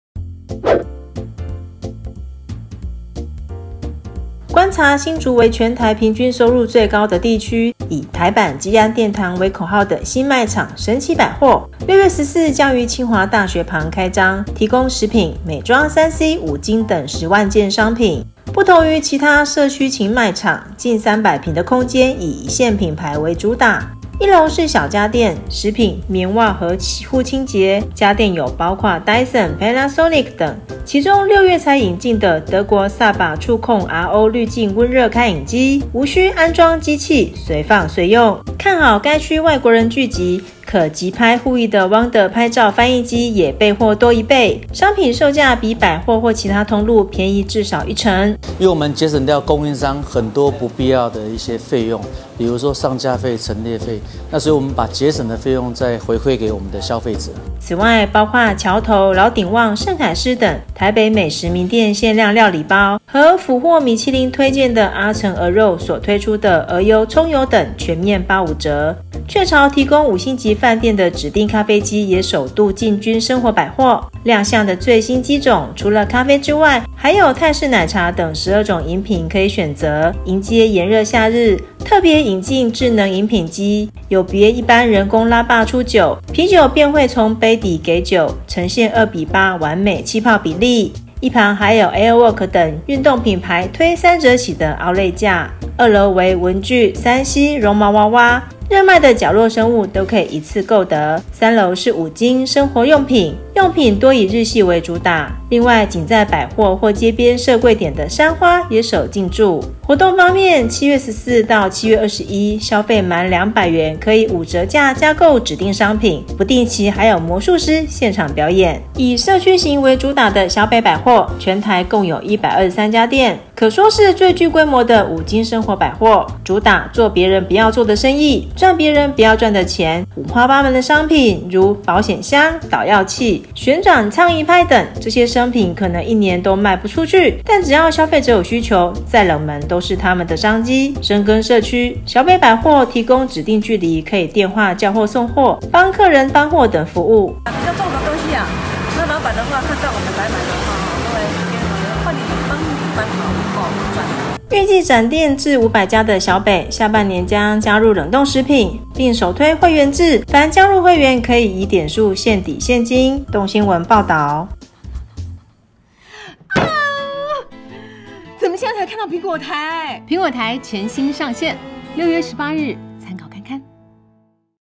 社區百貨比神奇 大品牌便宜1成 | 蘋果新聞網
除了超商、量販，許多人也會在家附近的平價生活百貨買東西，看好社區型百貨潛在的商機，以「台版激安殿堂」為口號的新賣場「神奇百貨」也將加入戰場，近300坪的空間，有食品、美妝、3C、五金等10萬件商品，以一線品牌為主打，一樓是小家電、食品、洗護清潔與棉襪，不乏Dyson、honey well、旺德等大品牌，神奇百貨負責人魏嘉伯表示， 店內少了不必要的上架、陳列費，所以售價將比百貨或其它通路便宜至少1成。其中，德國SABA觸控RO濾淨溫熱開飲機，擁有高效活性碳等四道濾心，無須安裝機器、隨放隨用，因應該店周圍外國人聚集，可即拍互譯的Wonder AI拍照翻譯機也備貨多1倍。此外，包括太和殿、老鼎旺、橋頭等台北美食名店限量料理包，與甫獲米其林推介的阿城鵝肉所推出鵝油蔥油等全面85折，比品牌自家餐廳還便宜。值得一提的是，雀巢提供五星級飯店的指定咖啡機也首度進軍生活百貨，最新機種除咖啡外，還有泰式奶茶等12種飲品，6/14~7/14全面買1送1。迎接炎熱夏日，特別引進智能飲品機，有別一般人工拉吧出酒，啤酒從杯底給酒，呈現2:8完美氣泡比例。2樓為文具、3C、參考書與絨毛娃娃，3樓是五金、生活用品，用品多以日系為主打，僅在百貨或街邊設櫃點的三花也首進，另不定期還有魔術師現場表演與消費者近距離互動，預計下半年將於台北與南台灣展店。